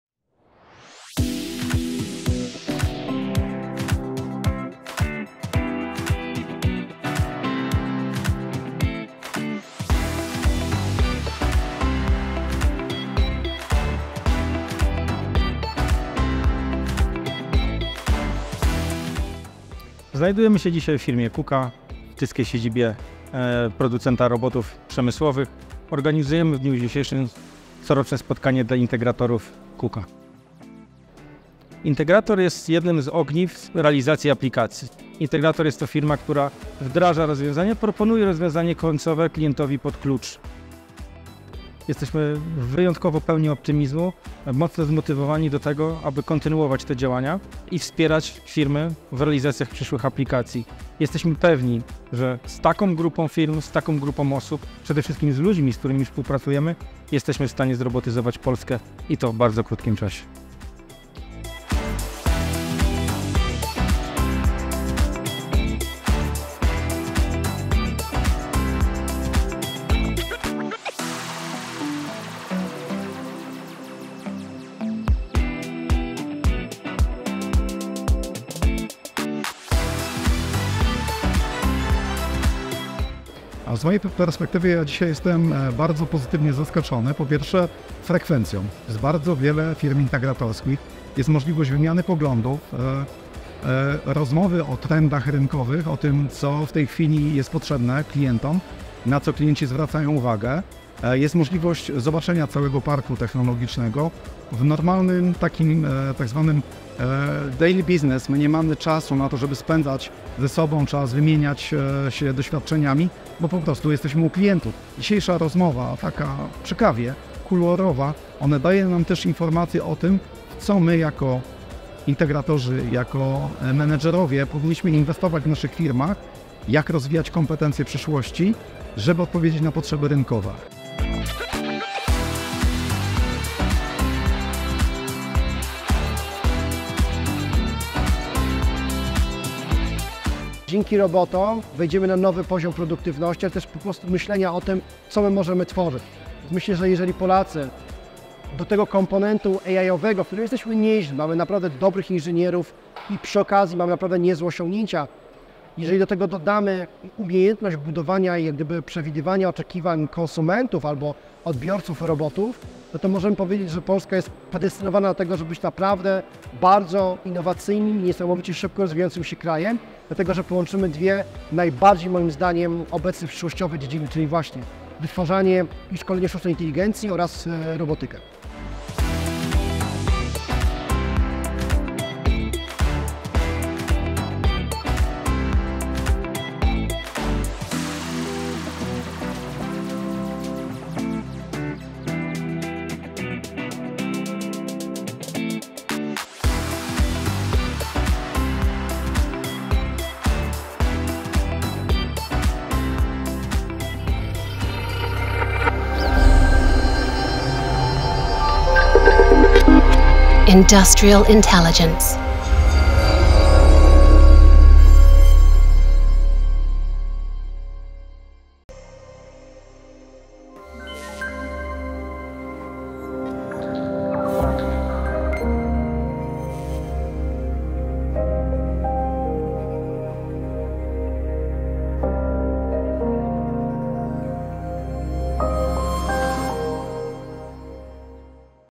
10th Integrators Meeting. Together we create the industrial future
The goal of the KUKA Integrators Meeting, which has been organized by KUKA Polska annually for the past 10 years, is to foster integration within the Polish industry, particularly among enthusiasts of automation and robotics. This gathering serves as a platform for exchanging knowledge, experiences, and insights.

At the 10th anniversary edition of the KUKA Integrators Meeting, a broad spectrum of companies and individuals with a vested interest in robotization were in attendance. This included not only regular system partners but also a range of other integration companies, expanding the reach and impact of KUKA's robotization initiatives.

The two-day event featured a rich program comprising in-depth lectures, practical workshops, demonstrations of robots and their solutions, and debates led by experts and industry leaders. Additionally, there were opportunities for networking, allowing participants to connect and share ideas.

Significant contributions to the event were made by various guests and partners, including Siemens Polska, EUCHNER GmbH + Co. KG, FlexLink Systems Polska, Industrial Robotics Company, Kemppi, SCHUNK Intec Polska, Techvitas, Beckhoff Automation Polska, Piab, Troax Poland, wenglor sensoric group, SICK Sensor Intelligence, Balluff Polska, and voestalpine Böhler Welding.

The organizers extend an invitation to view the report from the 10th KUKA Integrators Meeting, capturing the essence and highlights of this significant industry event.